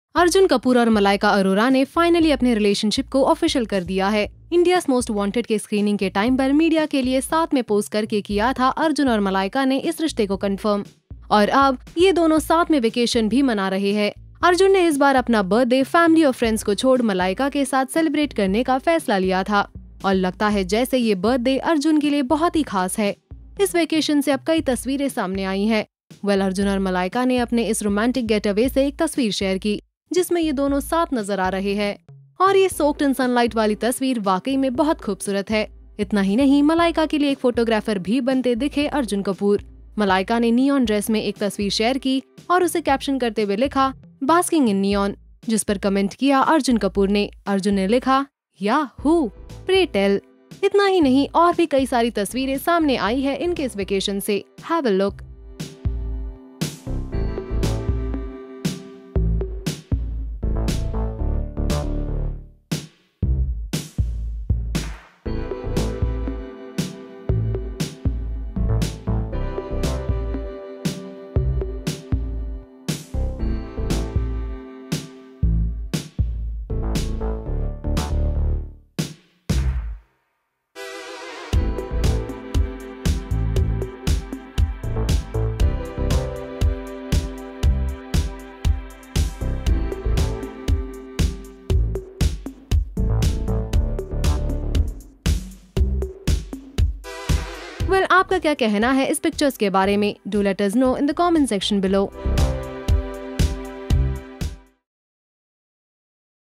Arjun Kapoor And Malaika Arora’s Vacation Pictures In NEW York Go Viral
Arjun Kapoor and Malaika Arora are enjoying their vacation in New York. And now their pictures are going viral. Have a look at some of their latest vacation pictures.